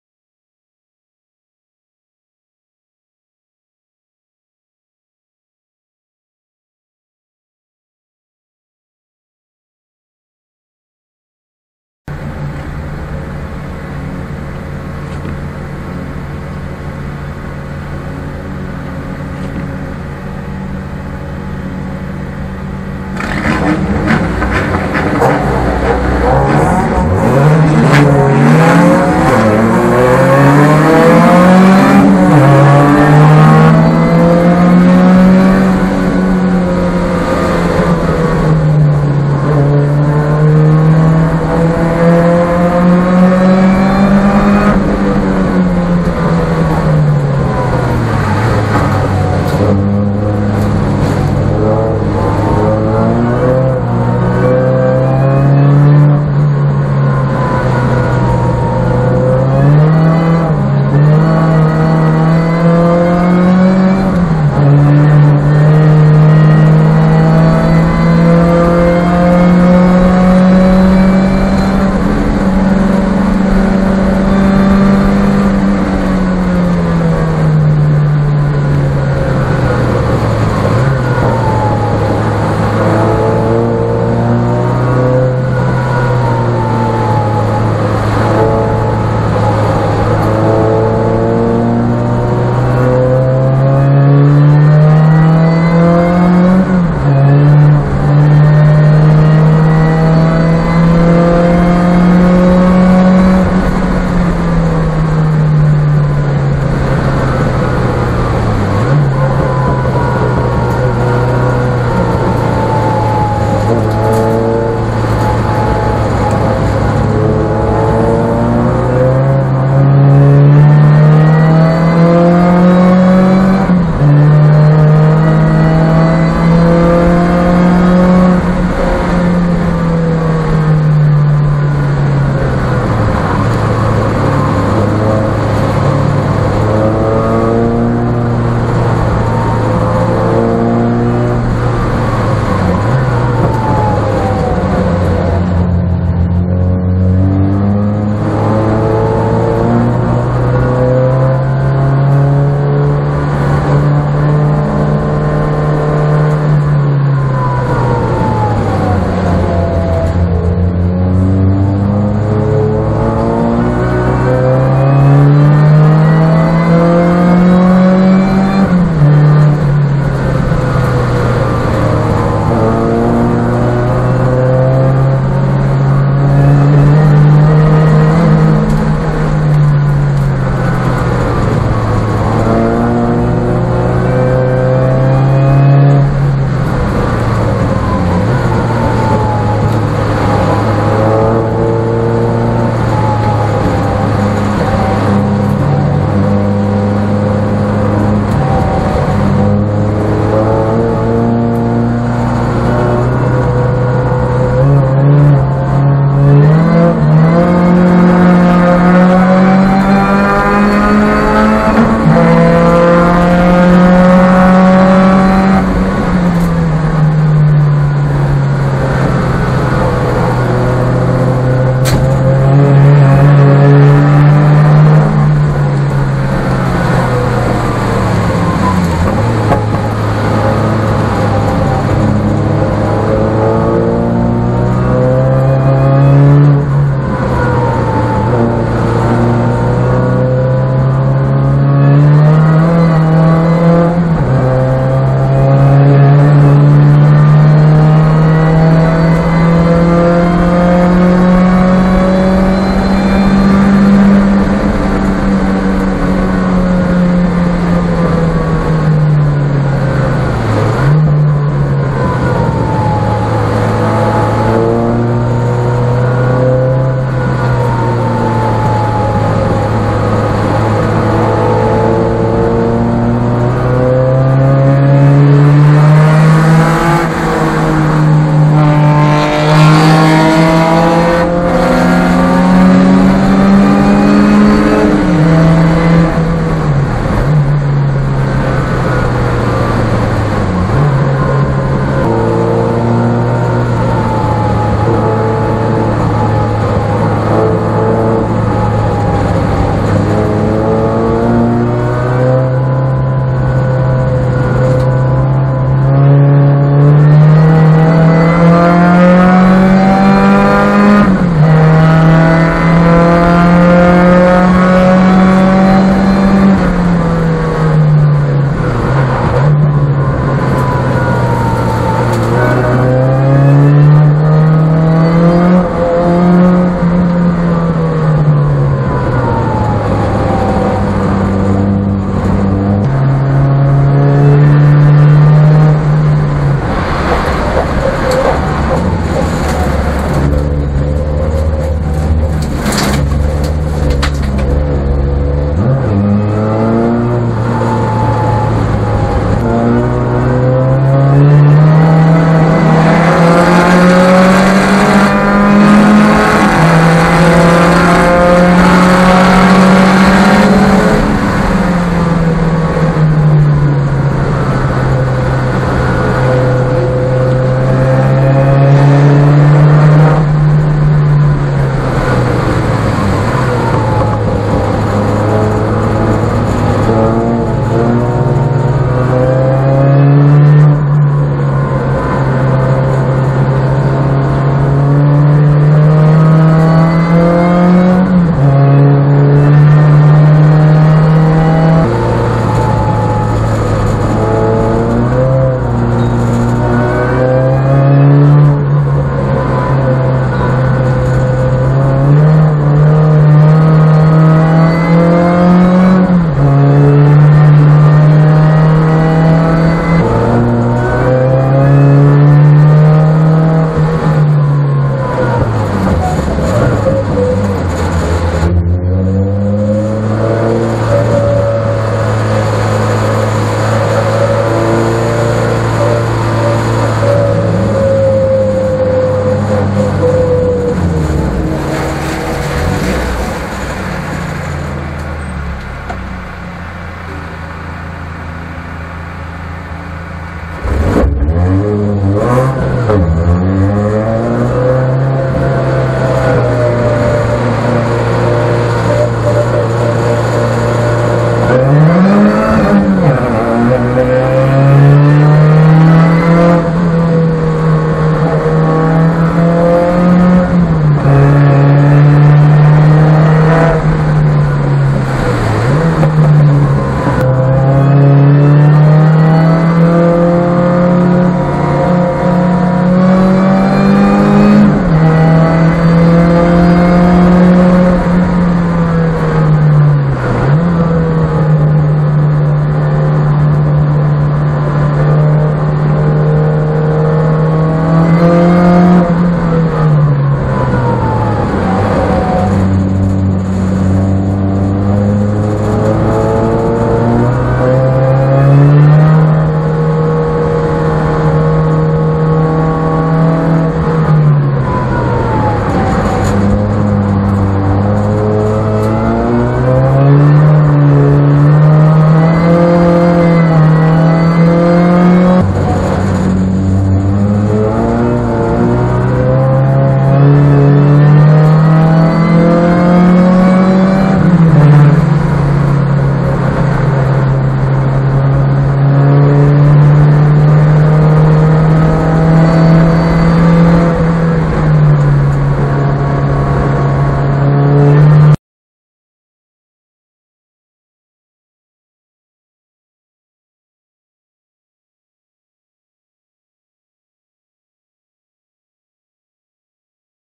super production challenge calabogie 2016 course 3 et 4
course 3 et 4 du super production challenge a calabogie motorsport du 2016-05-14 eric durand sur chevrolet cobalt ss/sc no 12 départ 3 course 3 fini 3 course 4 fini 4  course sous la pluie